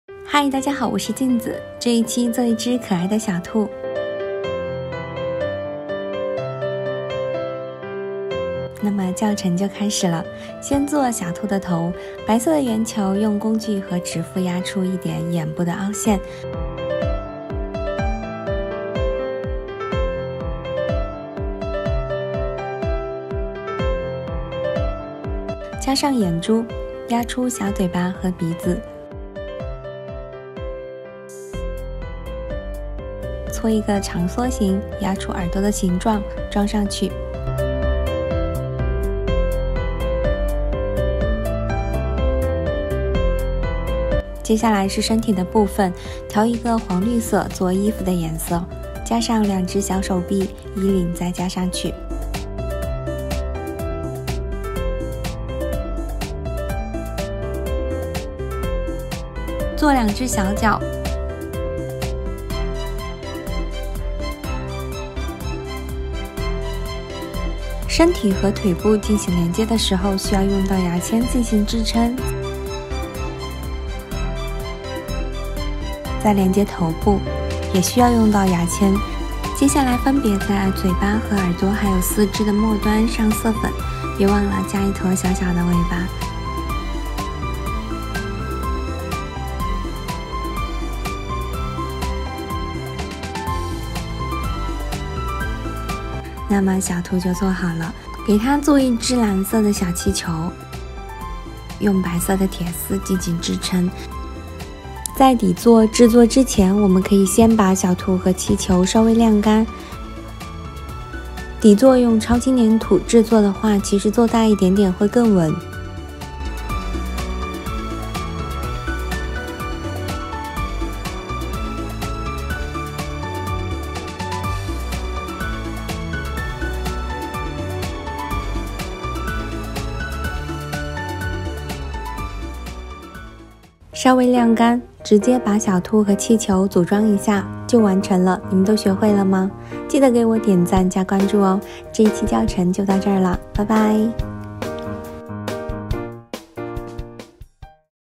DIY小兔和气球【动物粘土教程】How to make a rabbit and a balloon wih clay | Simple Animal Clay Tutorial
In this video, I made  a rabbit and a balloon with super light clay, the tutorial is detailed and simple. I hope I can teach you all kinds of cute animals, Do not forget to 'comment' and 'like' to me,thank you!